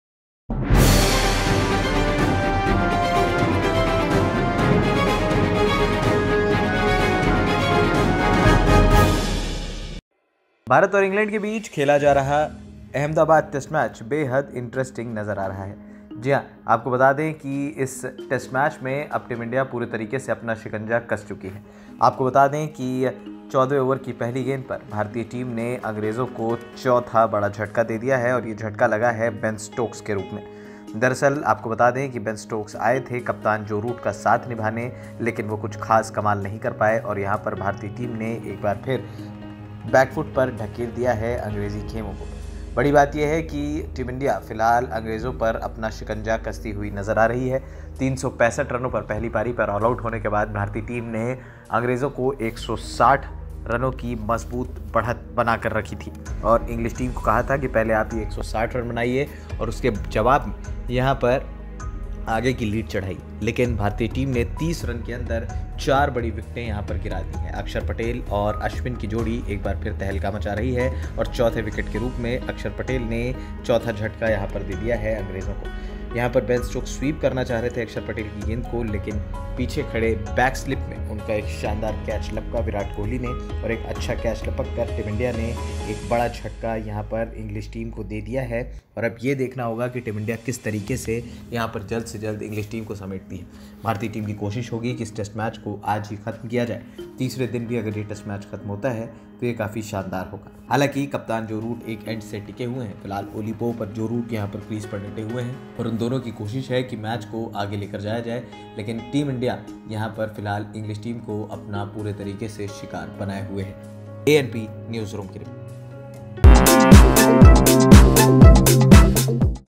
INDvsENG4thTEST:अंग्रेजों को लगा चौथा झटका,Stokes को Axar ने किया OUT,देखें कैसे गिरा विकेट?
इस ख़ास रिपोर्ट में देखें अंग्रेजों को लगा चौथा झटका,Stokes को Axar ने किया OUT,देखें कैसे गिरा विकेट?

ANPNEWS
anpnews